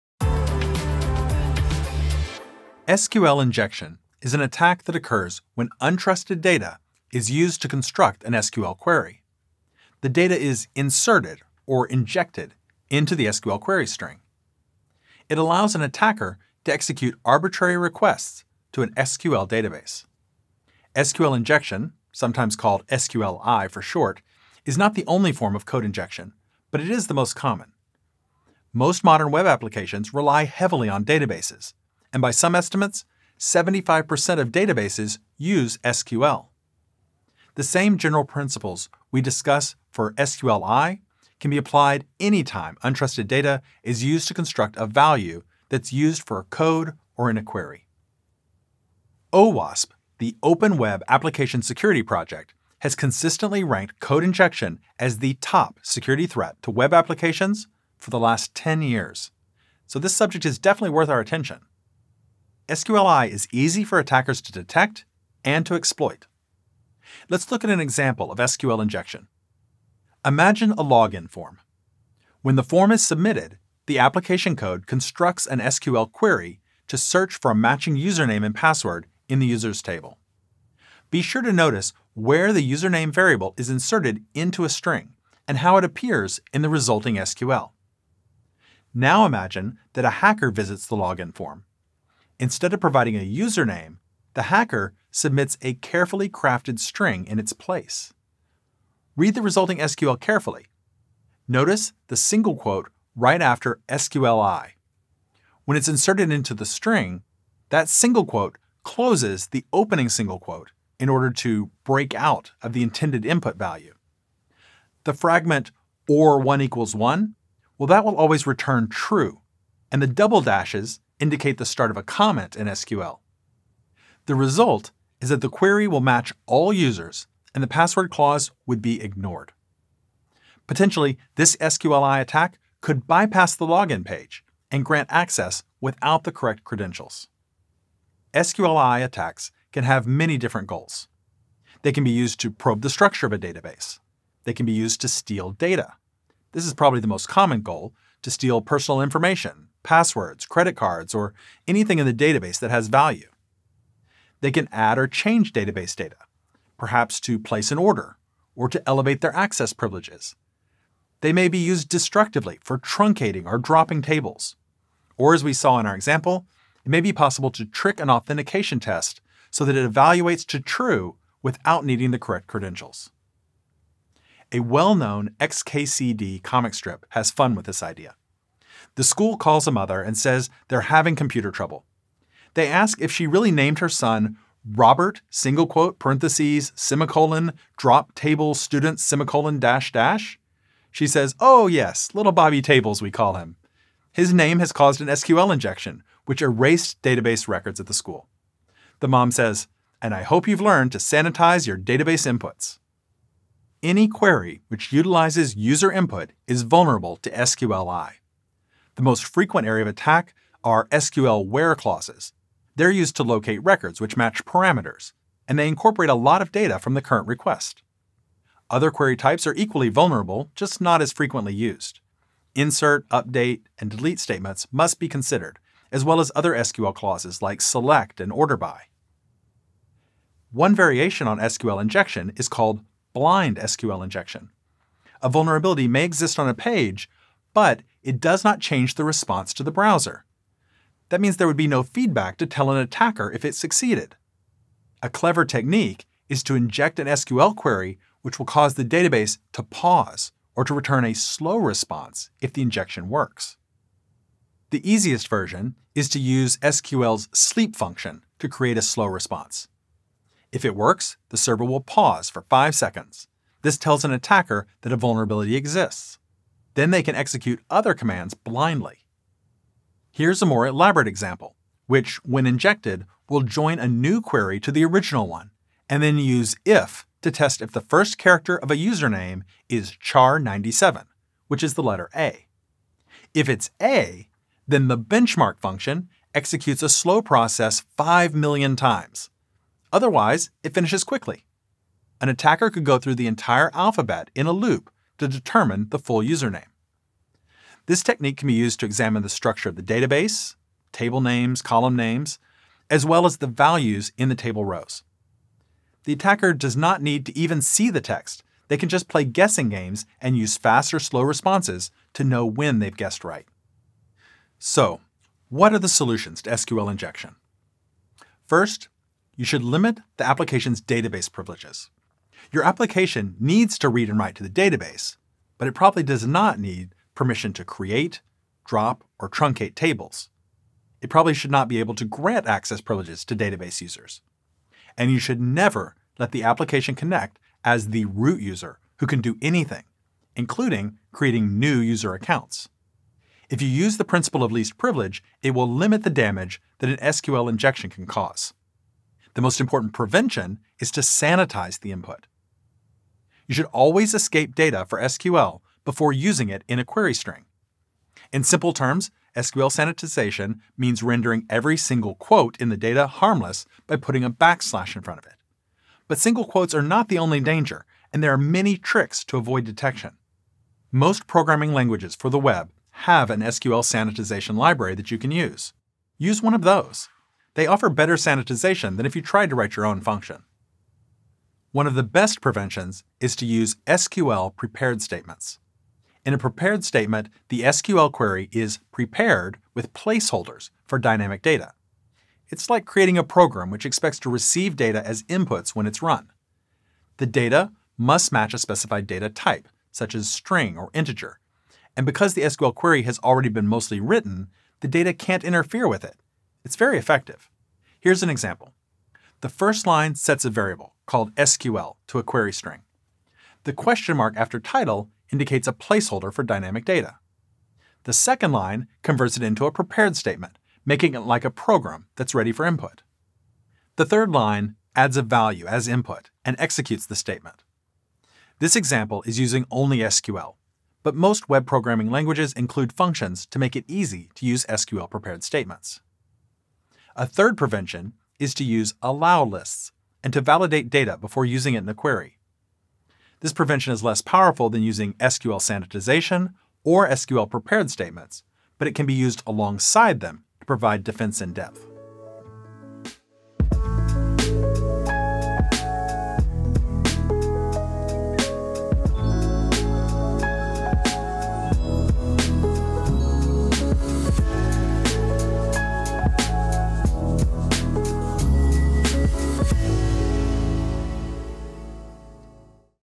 IT Security Tutorial - Preventing SQL injections
Learn about the risks that SQL injections can pose to your databases and what steps you can take to prevent them. Explore more IT Security courses and advance your skills on LinkedIn Learning:

This is an excerpt from "Programming Foundations: Web Security," a course on LinkedIn Learning taught by Kevin Skoglund. Kevin is the founder of the web development agency Nova Fabrica.